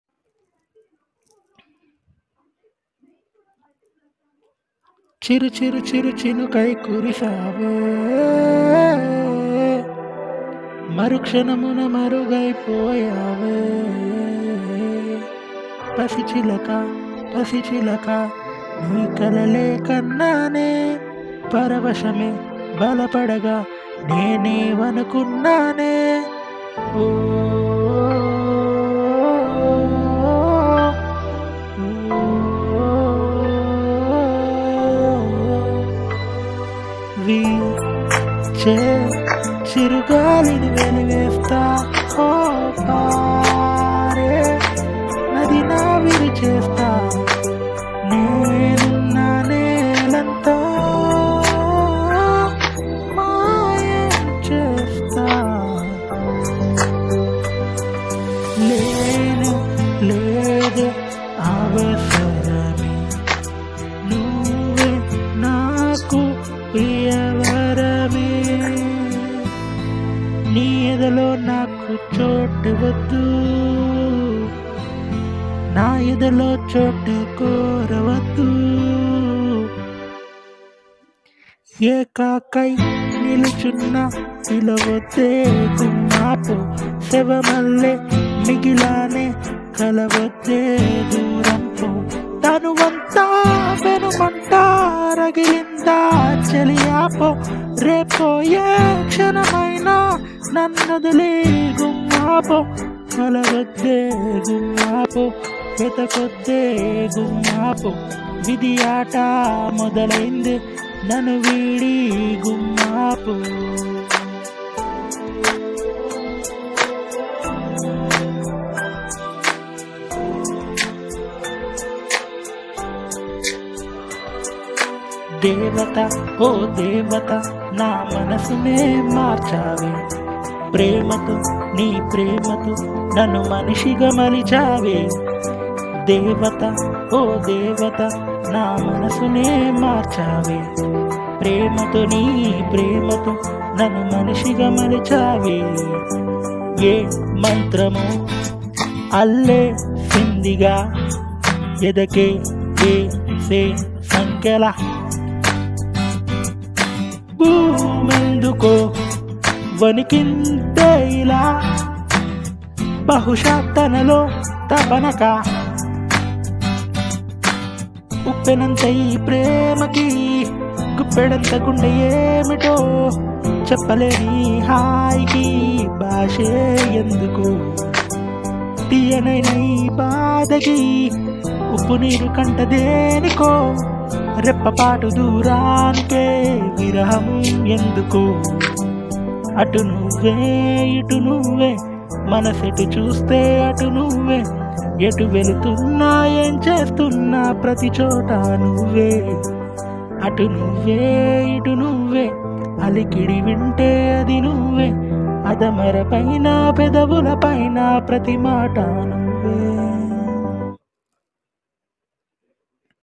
Telugu Songs Mashup (Karaoke)
Telugu best songs mashup sung by me using Starmaker app...